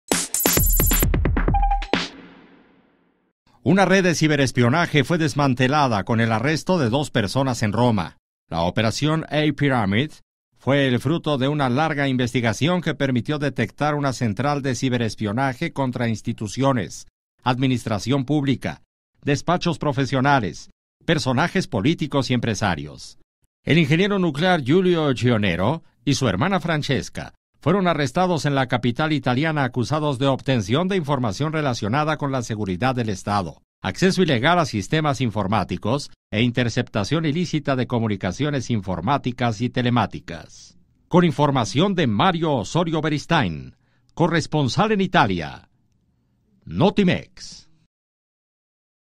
Desmantelan en Italia red que espiaba a políticos y funcionarios
Una red de ciberespionaje fue desmantelada el 9 de enero con el arresto de dos personas en la capital italiana